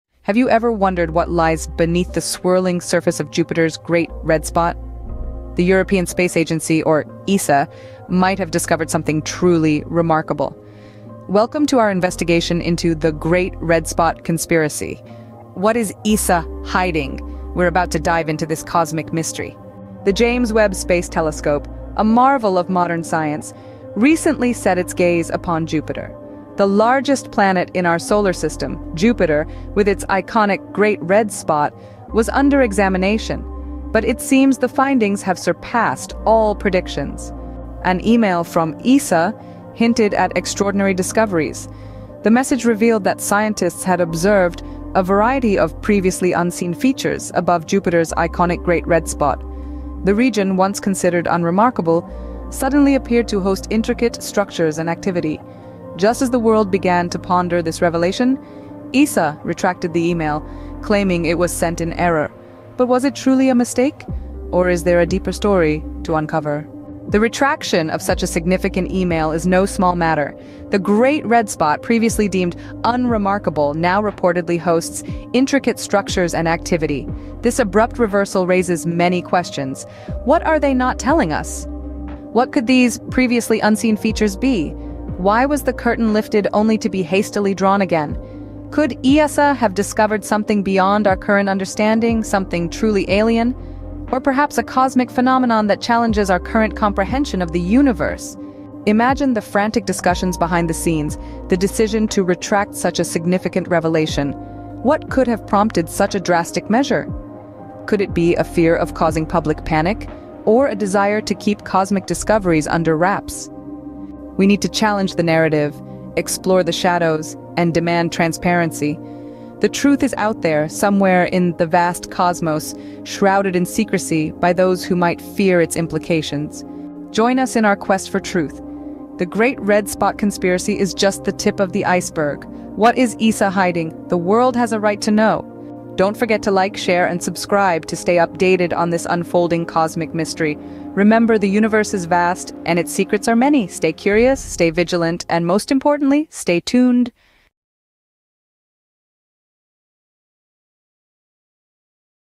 Jupiter Mystery! STRANGE SHAPES IN GIANT RED SPOT - What has James Webb Telescope Discovered?
It's not every day that an agency like ESA sends out an email like this and then pulls it back. We're talking about a region in space that was once considered 'unremarkable.' Now, suddenly, there are 'intricate structures and activity'? This isn't just a small oversight. There's something they're not telling us."What are these 'previously unseen features' mentioned in the first email? Why was the public's attention drawn to them, only to be shrouded in mystery moments later? Is it possible that the ESA stumbled upon something... otherworldly? Or perhaps, a cosmic phenomenon that challenges our understanding of the universe?"

Imagine the chaos behind closed doors, the hurried discussions, the decision to retract the revelation. What could prompt such a drastic measure? Is it the fear of public panic, or the desire to keep cosmic discoveries a secret?"
We must question the narrative, probe the shadows, and demand transparency. The truth is out there, hidden in the depths of space, veiled in secrecy by those who fear its implications."

Join us as we continue to investigate, to question, and to seek the truth. The Great Red Spot Conspiracy is only the beginning. What is ESA hiding? The world deserves to know."

Remember, the cosmos is vast, and its secrets are many.